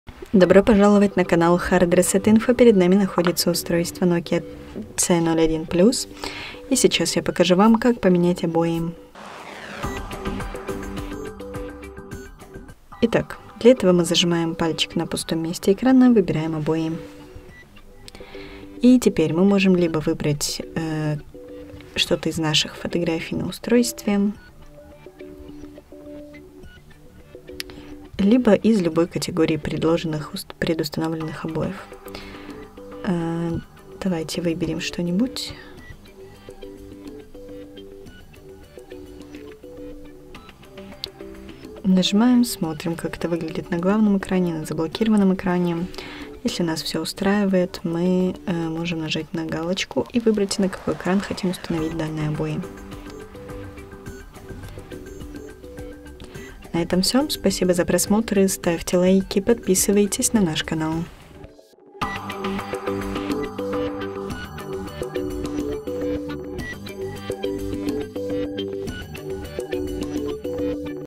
Nokia C01 Plus | Как поменять обои на Nokia C01 Plus - Как изменить заставку экрана на Nokia C01Plus
Личное оформление на максимуме: Как поменять обои на Nokia C01 Plus. В этом видео мы покажем вам, как легко обновить внешний вид вашего Nokia C01 Plus с помощью новых заставок. Узнайте, как изменить обои на своем Nokia C01 Plus и придать ему индивидуальный стиль!

Как поменять обои на Nokia C01 Plus? Как настроить заставку экрана на Nokia C01 Plus? Как изменить заставку Nokia C01 Plus?